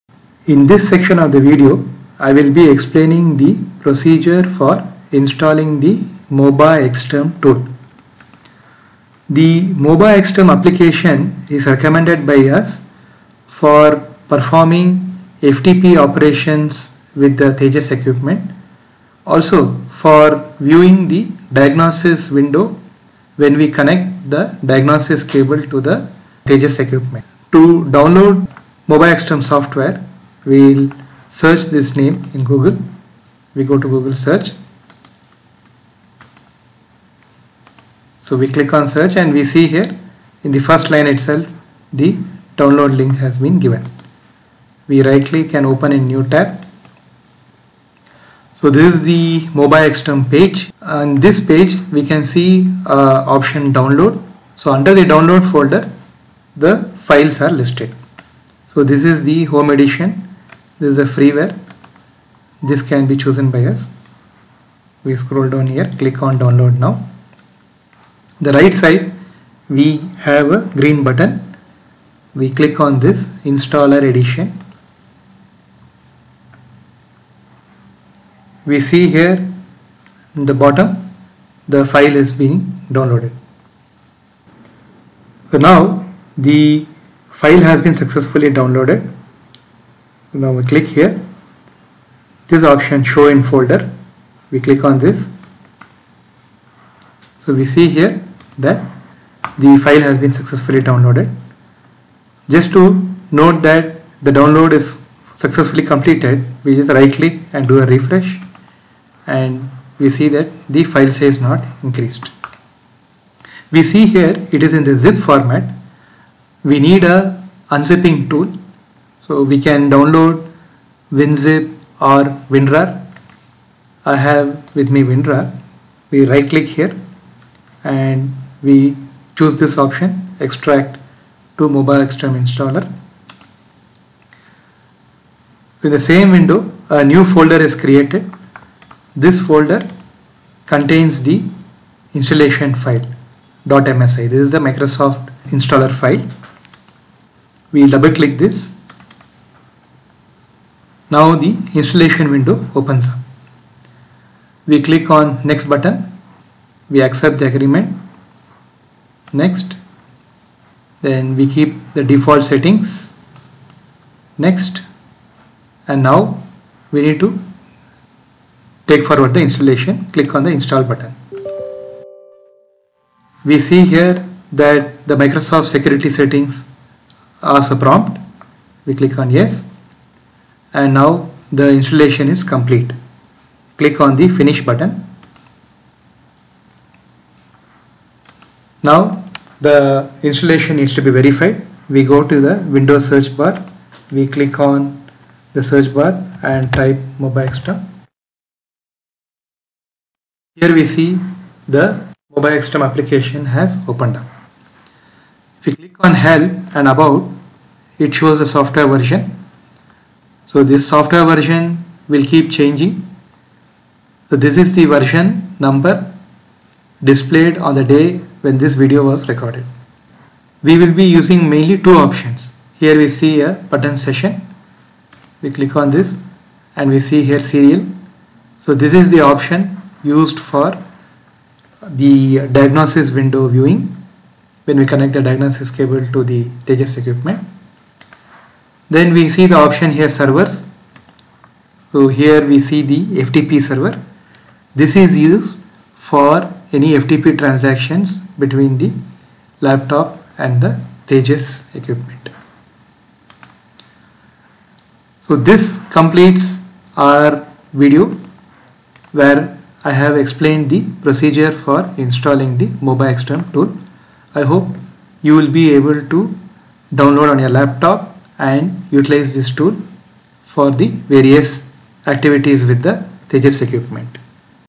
MobaXterm Download and Install Procedure
we suggest downloading and using the freeware 'MobaXterm'.
This demo version of the tool has excellent features (Command windows, FTP server, telnet, Serial communicator, etc.)